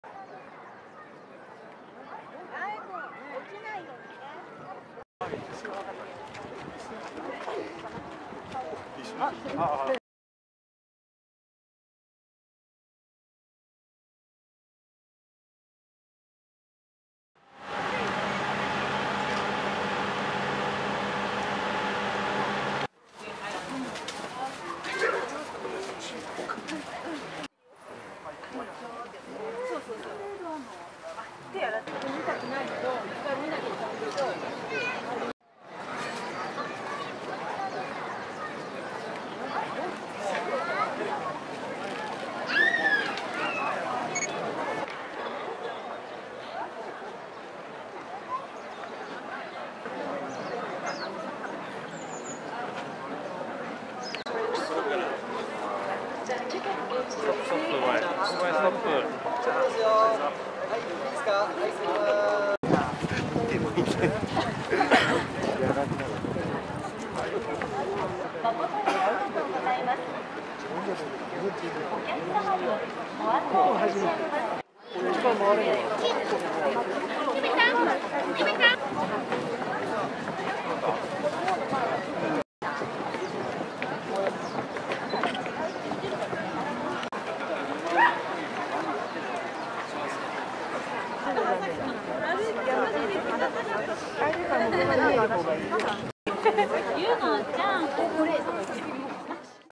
高遠城址公園の桜見ごろ
長野県伊那市の高遠城址公園でタカトオコヒガンザクラが見ごろになりました。１５００本が植わる園内は満開で、花は近年では一番の多さといい、山は一面ピンク色に染まっていました。

園内は、桜やや中央アルプスの雪山を眺めたりする多くの人たちで混雑していました。進徳館（旧藩校）や桜雲橋、問屋門
、太鼓櫓などと桜の景色が美しかったです。